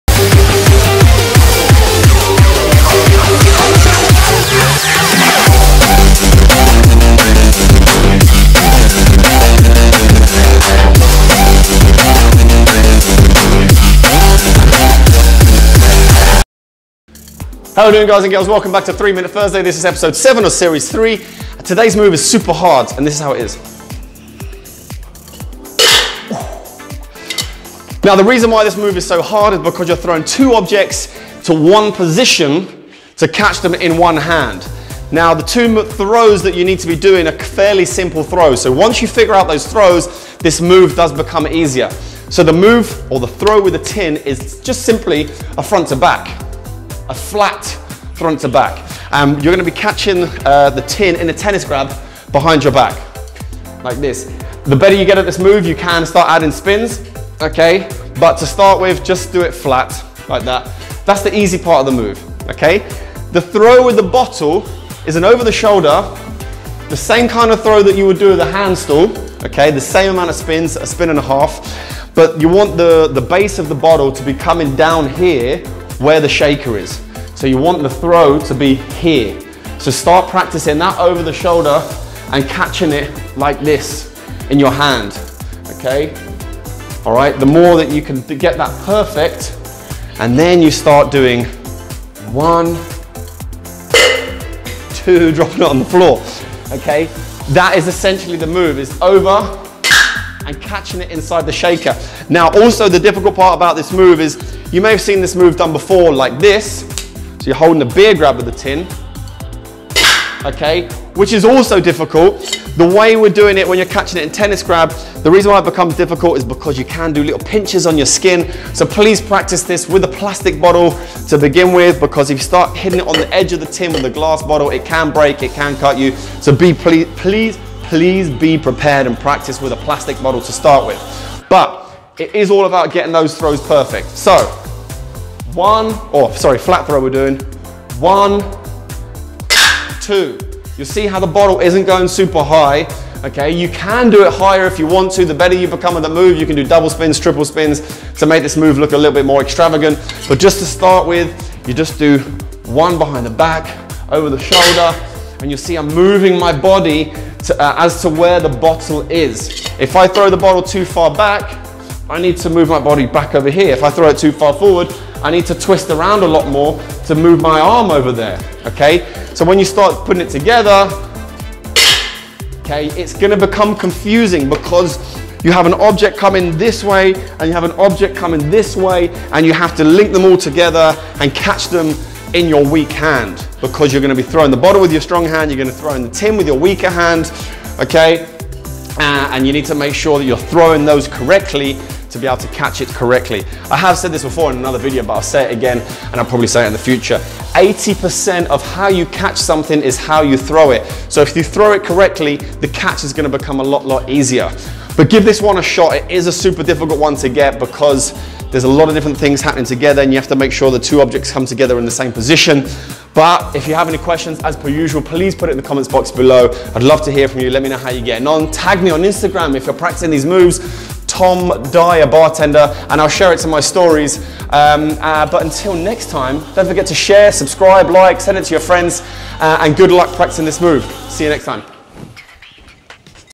The Magnet - 3minThurs s3e7
How we doing guys and girls welcome back to Thursday, this is episode seven of series three.  Today's move is super hard and this is how it looks.  The readson this move is so hardis because you are throwing two objects to one position to catch them in one hand.

The two throws that you need to be doing are fairly simple throws so once you figure out those throws and the move does become easier.